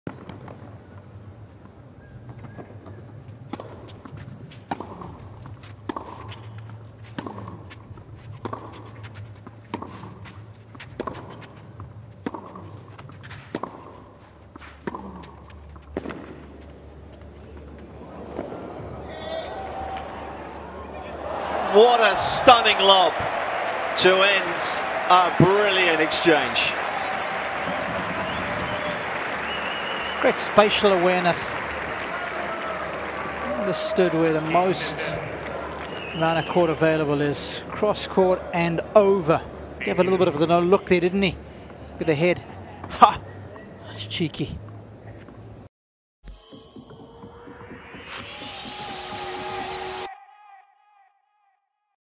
Fognini Fine Touch In Madrid 2016 Hot Shot
Fabio Fognini rounds off a lengthy exchange against Kei Nishikori with a lob winner on Wednesday at the Mutua Madrid Open. Watch live tennis from Madrid on TennisTV.com. Photo: Getty Images